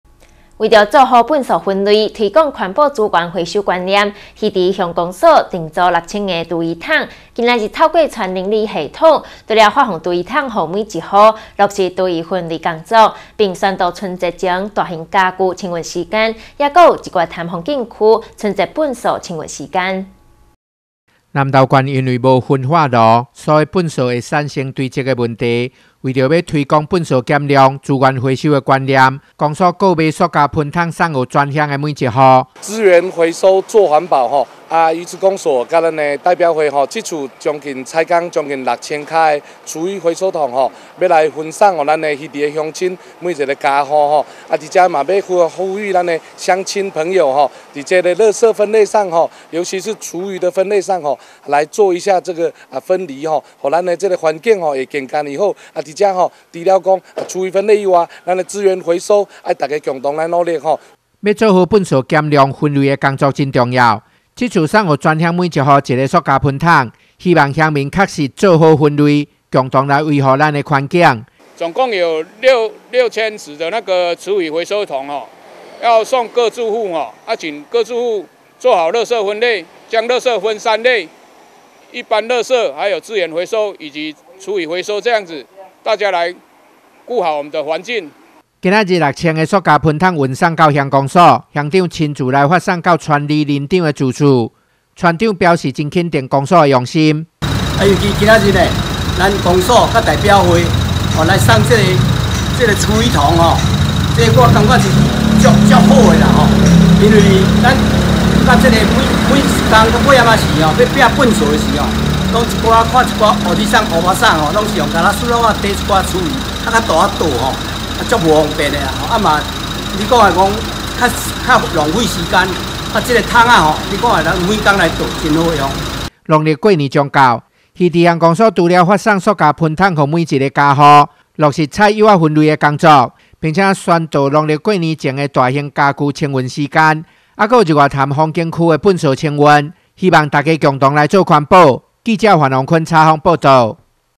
南投新聞 魚池公所發放六千廚餘桶
為了做好垃圾分類，推廣環保資源回收觀念，魚池鄉公所訂製六千個廚餘桶，今天透過村鄰里系統，除了發放廚餘桶給每一個家戶，落實廚餘分類工作，並且宣導春節前大型傢俱清運時間，還有日月潭風景區春節垃圾清運時間